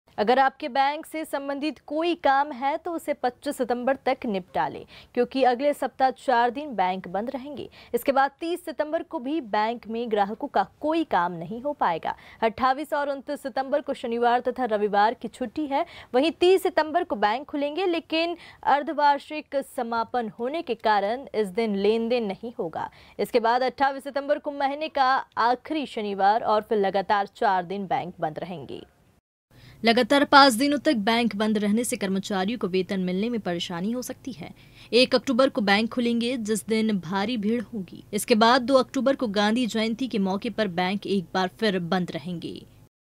Indore लगातार बैंक बंद
अगर आपके बैंक से संबंधित कोई काम है तो उसे 25 सितंबर तक निपटा लें, क्योंकि अगले सप्ताह चार दिन बैंक बंद रहेंगे। इसके बाद 30 सितंबर को भी बैंक में ग्राहकों का कोई काम नहीं हो पाएगा 28 ओर 29 सितंबर को शनिवार तथा रविवार की छुट्टी है। वहीं, 30 सितंबर को बैंक खुलेंगे, लेकिन अर्द्धवार्षिक समापन होने के कारण इस दिन लेनदेन नहीं होगा। इसके बाद 28 सितंबर को महीने का आखिर शनिवार ओर फिर लगातार चार दिन बैंक बंद रहेंगे।